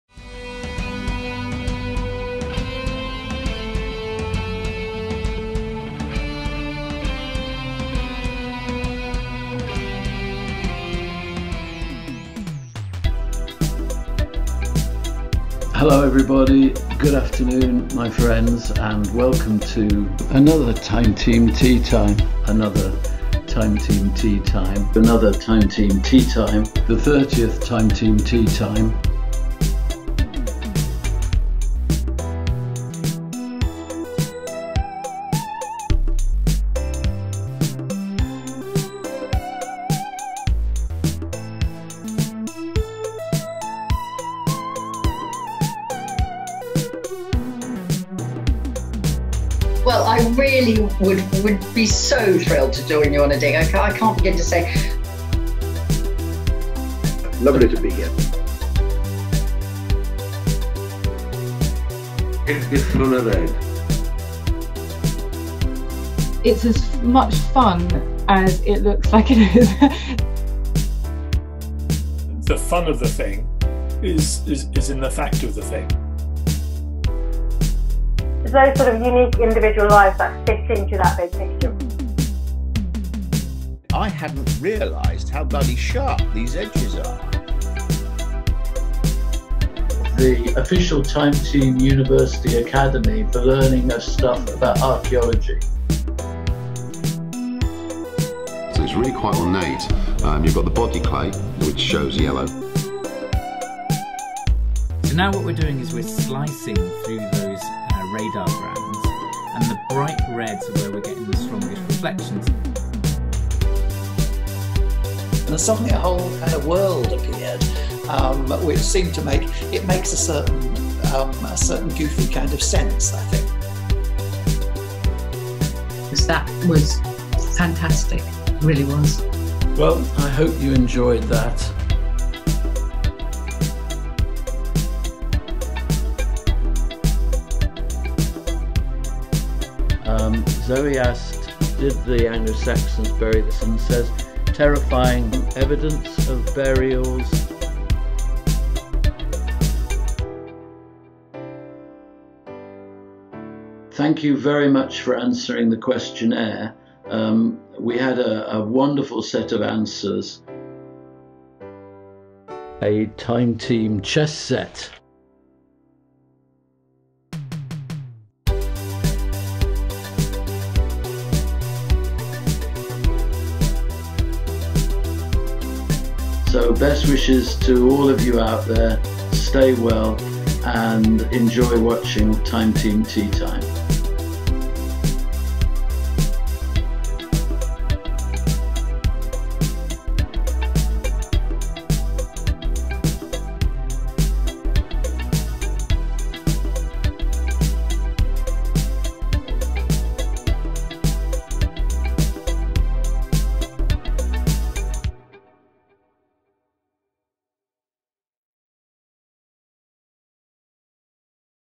Time Team Teatime 32: Best of Teatime
We relive some of our favourite Teatime moments, from comparing tools with Ken Follett and entering the Labyrinth with Kate Mosse, to catching up with Philippa Gregory and exploring the Last Kingdom with Bernard Cornwell.

To give you some inspiration, we'll be reposting links to a few highlights across our channels, so keep a look out.

We'd also love to hear from you about your own favourite moments in the comments below.

Thanks very much for your support, from Tim Taylor and all the team!

**TEATIME QUIZ**

Just for fun – how well do you know your Teatime sessions? Will you be crowned the ultimate Teatime superfan? Try to answer all of the following questions in the comments below.

Q1: Which Time Team regular won a 'long service medal' for filming 100 episodes?
Q2: According to Michael Morpurgo, what unusual historic artefact did a French school child discover in the garden?
Q3: Which artefact did Philippa Gregory fondly remember looking at on the Time Team shoot at Syon House?
Q4: Judging by an impressive Dalek collection, which Time Team expert is a massive Dr Who fan?
Q5: What did Time Team digger Jim Mower (who later became a producer on the show) consider to be the 'find of his lifetime'?

Good luck!!